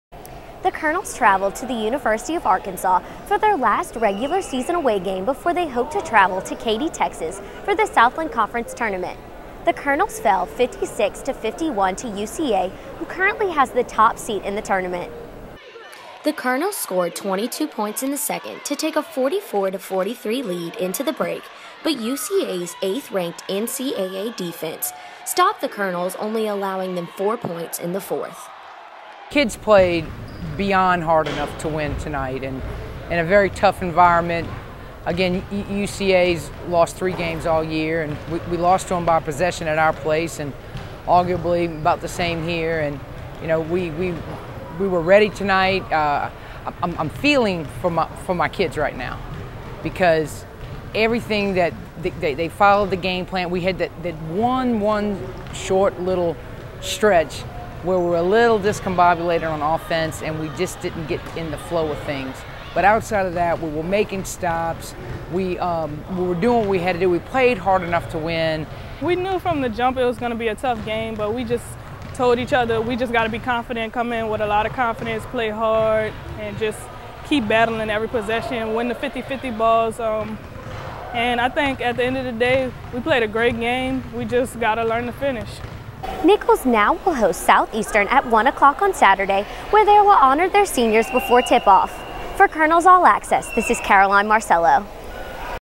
Nicholls @ University Of Central Arkansas WBB
Facing the league's toughest defense, the Nicholls State University women's basketball team had a one-point lead over Central Arkansas after three quarters, but the Sugar Bears held the Colonels to four points in the final period as Nicholls fell, 56-51, on March 3rd at the Farris Center.

The Colonels (9-18, 7-10 SLC) trailed 20-12 after the first, but responded by erupting for 22 points in the second to take a 44-43 into the break. Nicholls fell behind by as much as 12 in the first half, but ended the quarter with an 18-5 run.